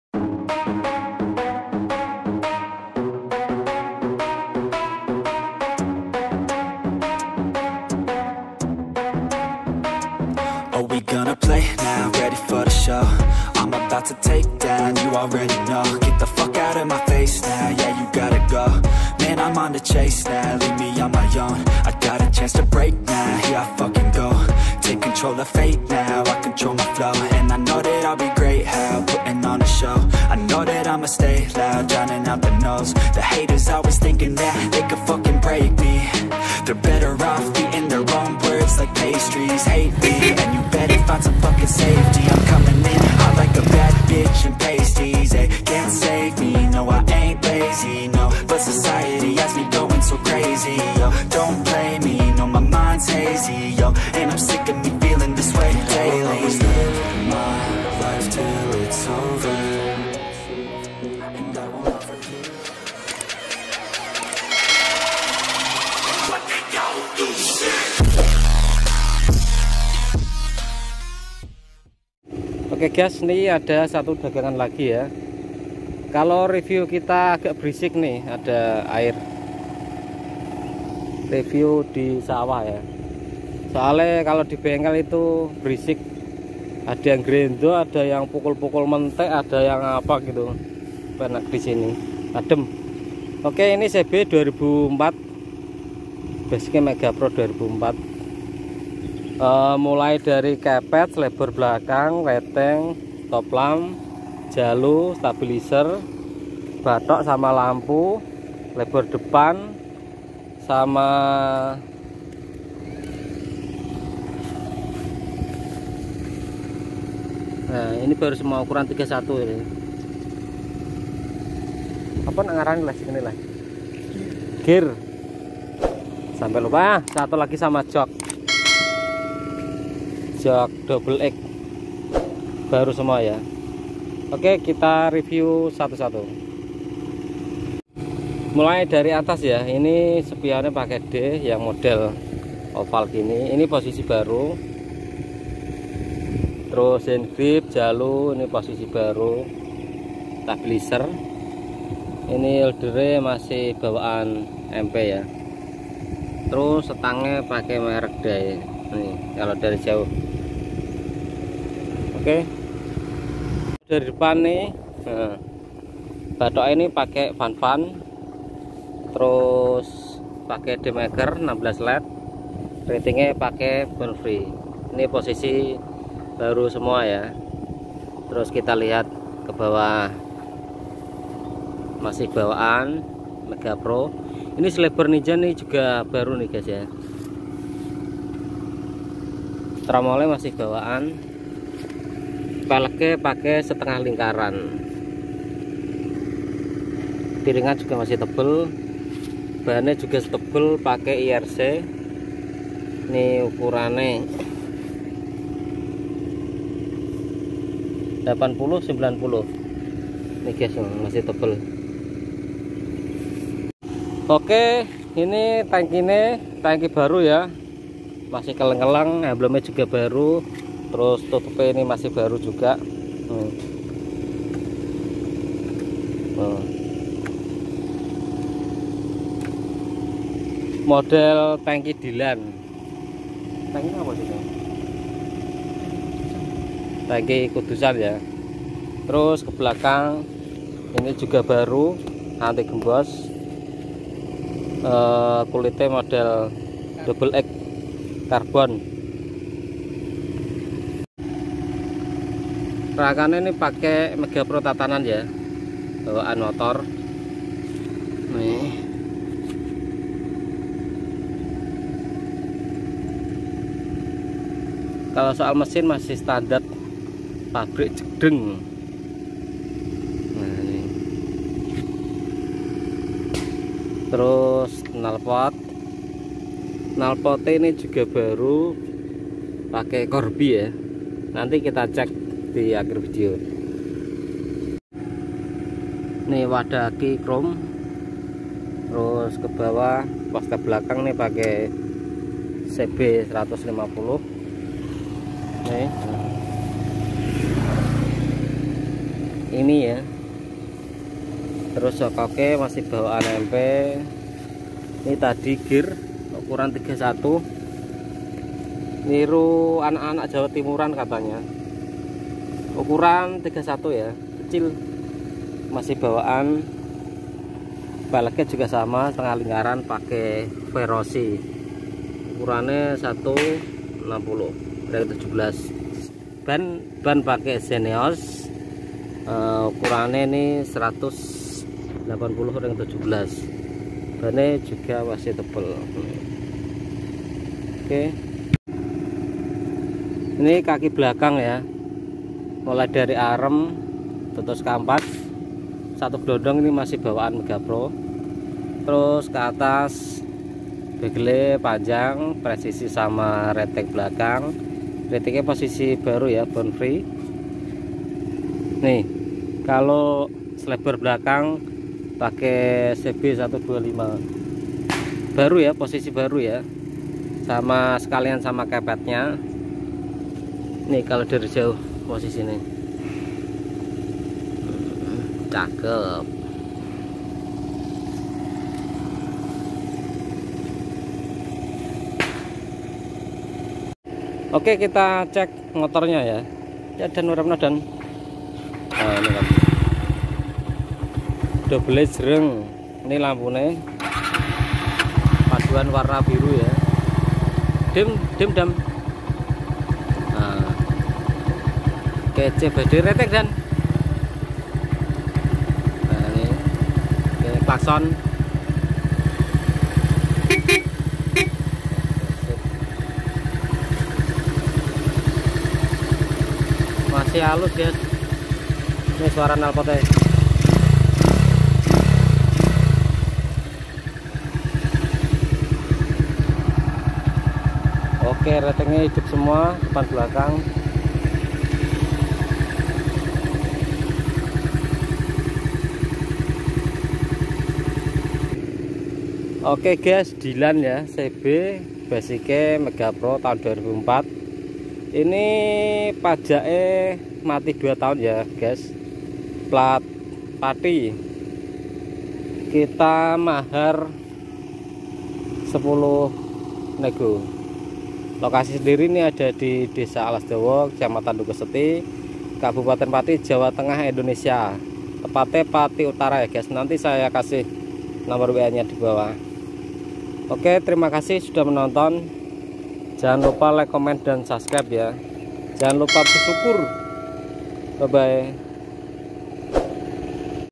di jual cb dilan MP 2004 | pak eko
Ok guyz.. ada dagangan lagi cb basik MP thn 2004... untuk spek harga dll bisa cek vidio di atas 🙏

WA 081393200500